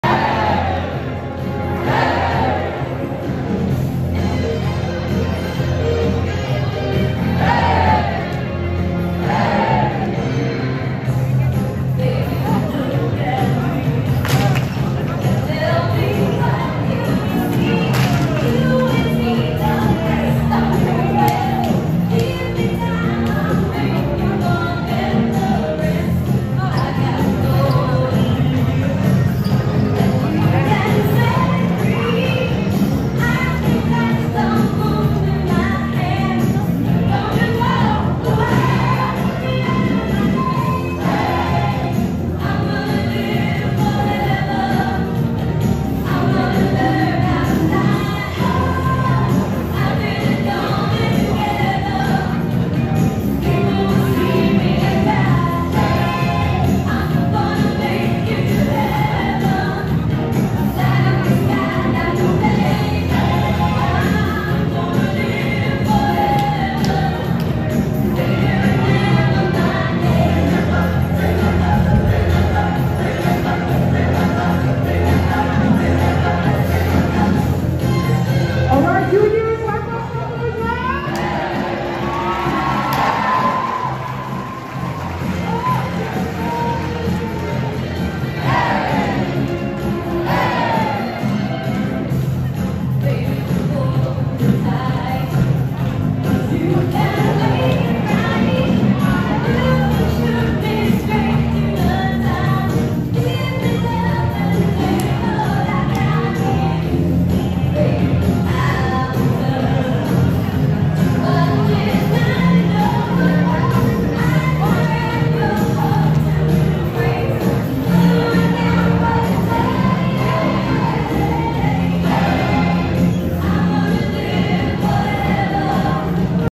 Class of 2025 - CLASS DANCE
Class of 2025 displaying their dance moves to their class song. Let's go!!